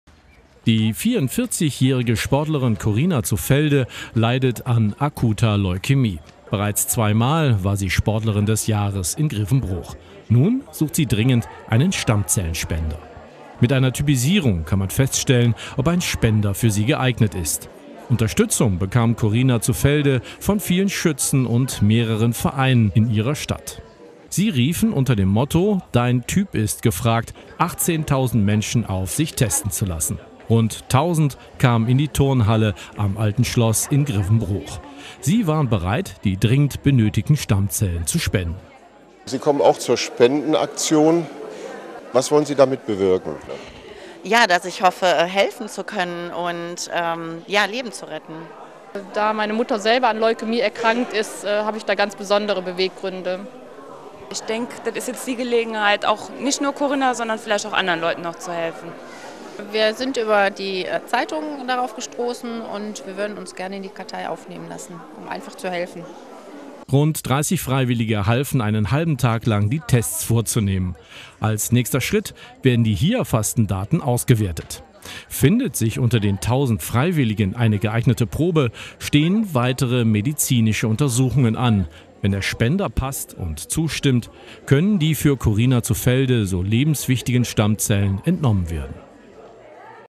Typisierungsaktion in Grevenbroich
Ganz viele Grevenbroicher haben sich zusammengetan und eine Typisierungaktion organisiert, um einen Spender zu finden. Die Aktion war am Wochenende - CityVision-Reporter Dieter Stanjek und Christoph Denkers waren dabei.